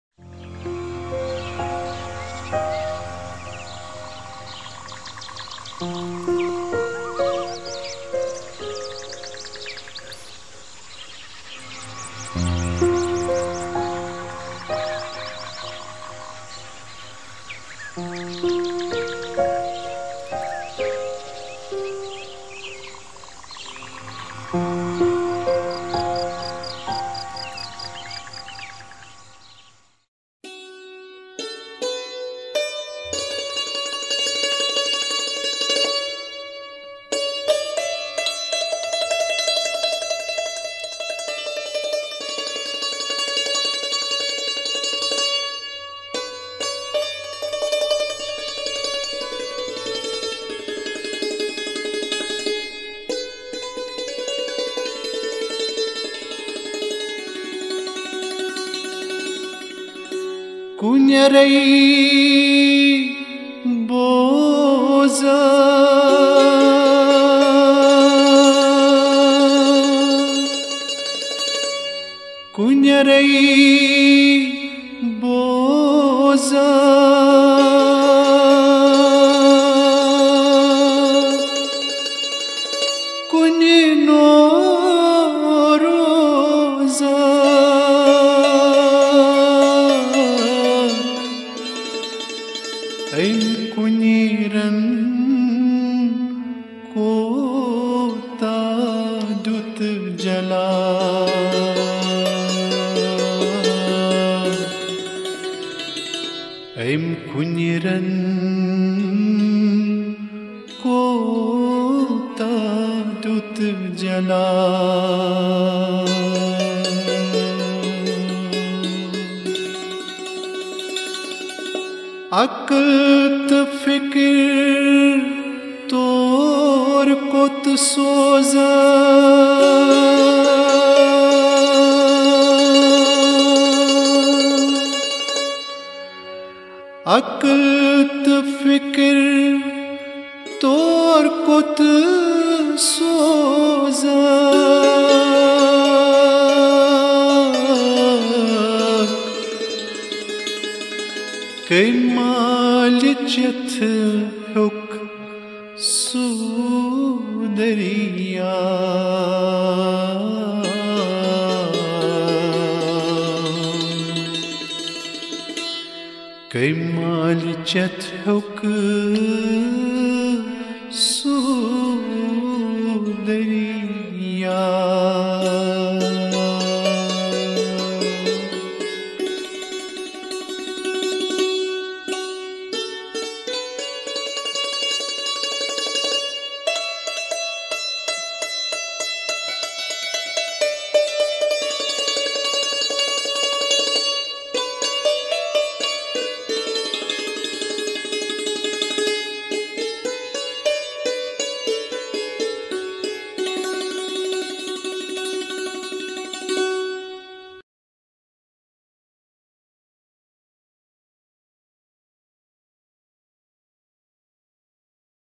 "SHRUKH" - SINGER AB RASHID FARASH-2 FROM RAVIMECH STUDIOS
THIS IS THE SECOND  PART OF OUR SERIES ON SHRUKH BY SHEIKH UL ALAM AND RENDERED BEAUTIFULLY BY LATE AB. RASHID FARASH. SANTOOR PLAYED BY LATE MUSHTAQ AHMED KALINBAFF. THIS IS FROM OUR ARCHIVE